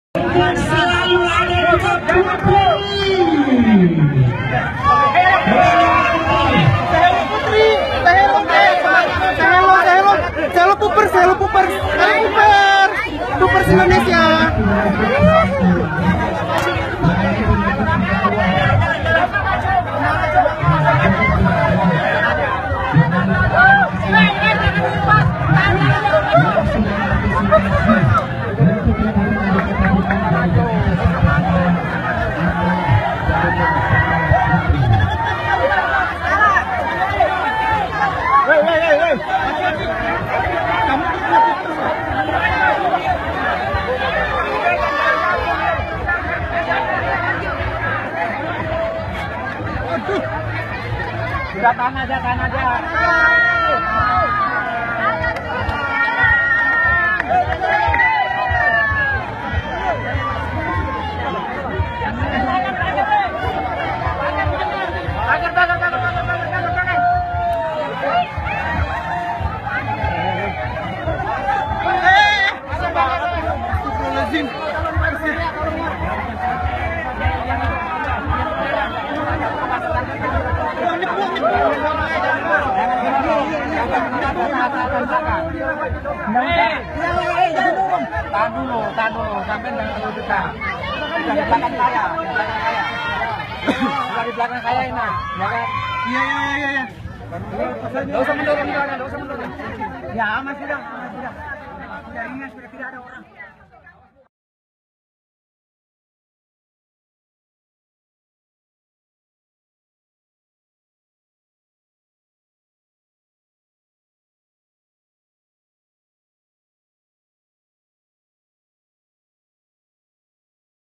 Kedatangan Putri Da4 di Balikpapan - disambut ribuan masyarakat Kota Balikpapan
Ribuan Masyarakat Balikpapan menyambut kedatangan Putri Da4 ...